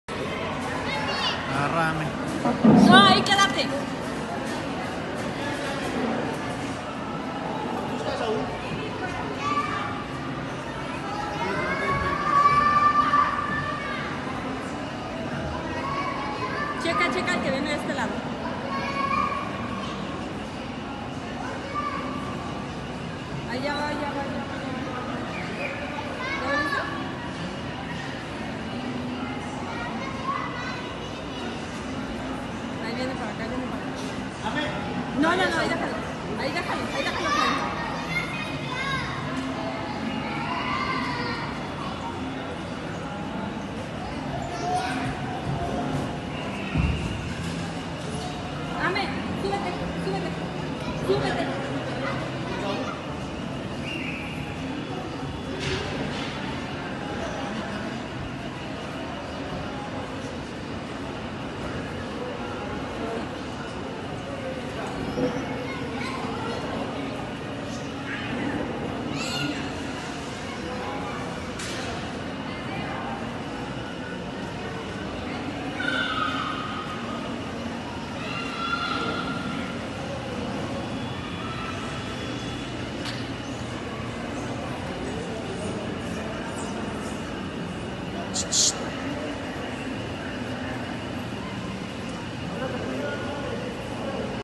Asalto a la MacStore en Galerías Celaya
Testigos del asalto a la tienda MacStore grabaron el momento en el que al menos 6 ladrones sustraen mercancía de la tienda. Autoridades ministeriales y de la policía municipal lograron la captura de 4 delincuentes, mientras que al menos 2 lograron escapar en un vehículo tipo Chevy color rojo.